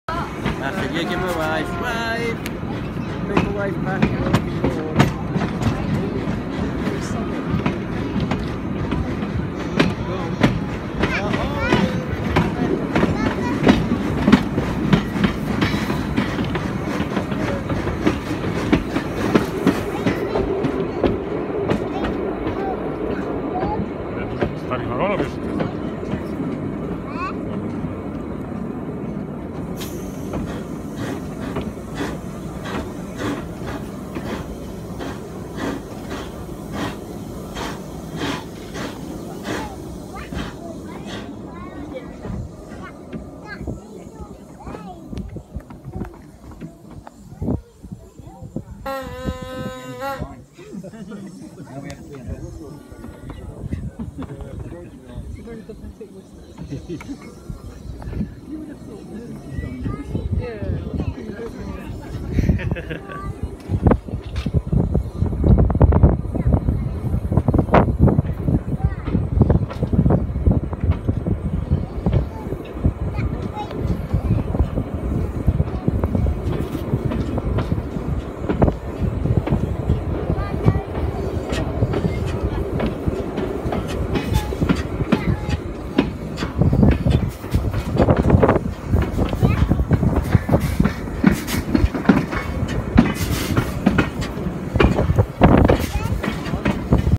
A ride behind the rocket at tyseley locomotive works
Here we pass 4965 rood Ashton Hall awaiting its next duty whilst waiting behind the shed in the background is 7029 clun Castle